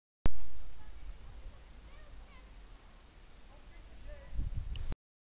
Molen Kijkduin te Schoorl
Net winterzeilen voorgehangen 26-09-09